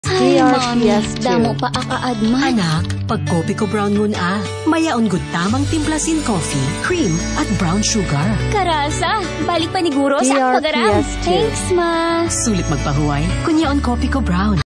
Kopiko Brown Coffee Radio Ad Q4 2022-2024 15s (Waray Version)
Ito po ang Q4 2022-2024 Radio Ad ng Kopiko Brown Coffee. (Waray Version)

DRPS Two 6264: Kopiko Brown Coffee Radio Ad Q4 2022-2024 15s (Waray Version)